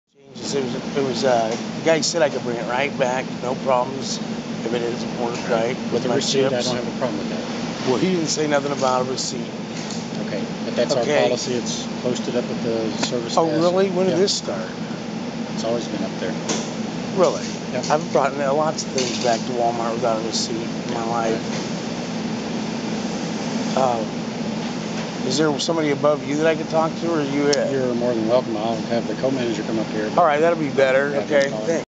I'm saying Council Bluffs iowa's cops are passing there three tear screen.
I've baited the entire force and I have to say they pass the Herksters heaviest screen yet, you might of all thought I was mad at you, even Jones, but i'm not, you all did good, you passed as being a Good Force, I baited you all so hard because Donna Dias from Hastings Ne, raped Rose Kelly, and got away with it, Doyle Dias did the raping of a 15 year old child, and adamd county covered it up.